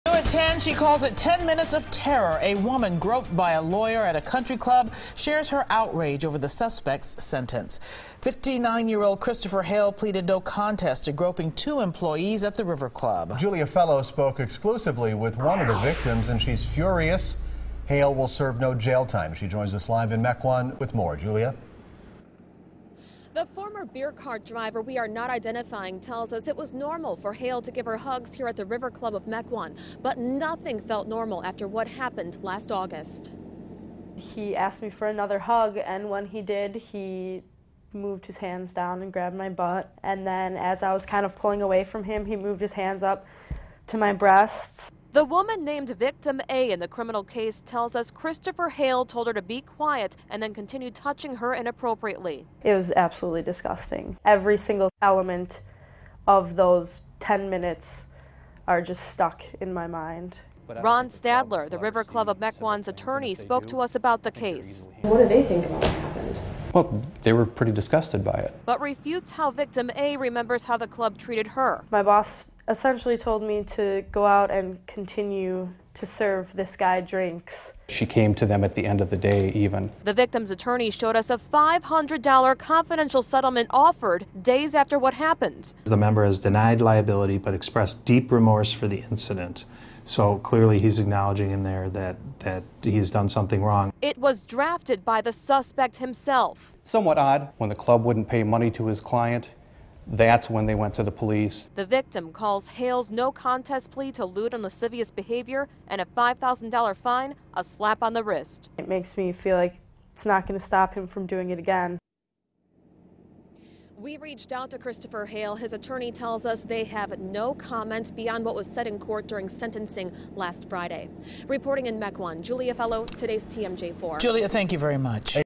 Milwaukee lawyer fined $5,000 for groping two female employees at his country club
Lawyer fined for groping country club employees